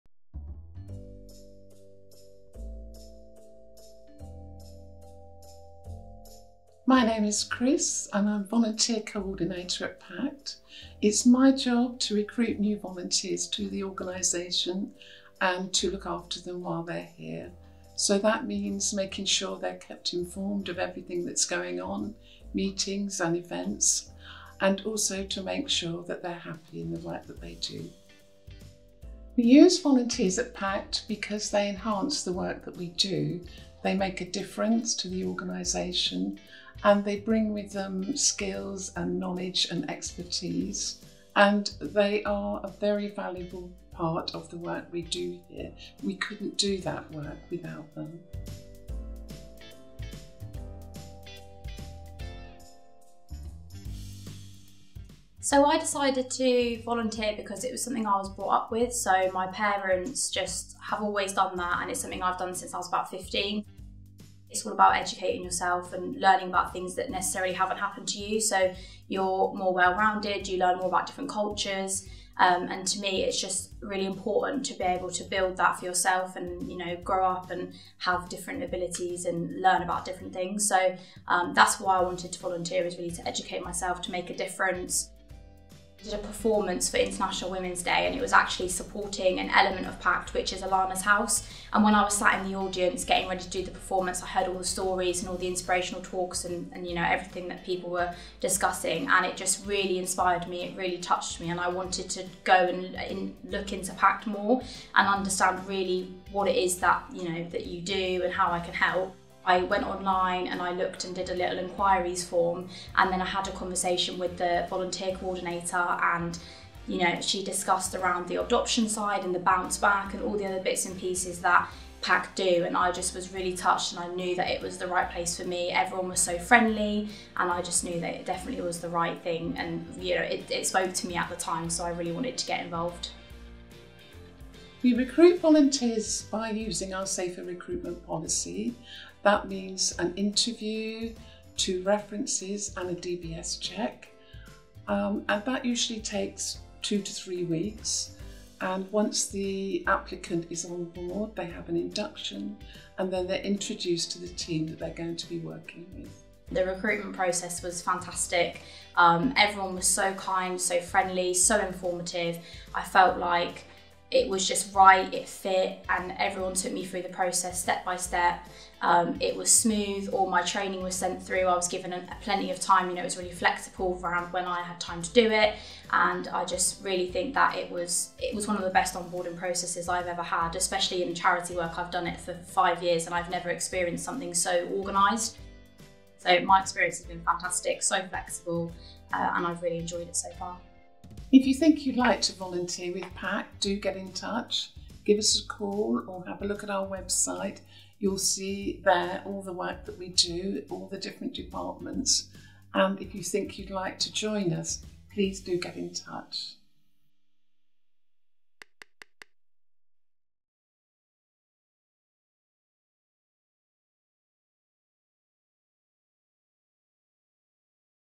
Volunteering at PACT | Become a volunteer today!
PACT is enormously grateful for the time, skills and talents given freely by the range of volunteers working with us today.

Both the families we work with and PACT staff benefit from the life experience and generosity that our volunteers bring to our projects and services.

By donating your time and energy to volunteer with PACT, you can help us build and strengthen forever families for the future.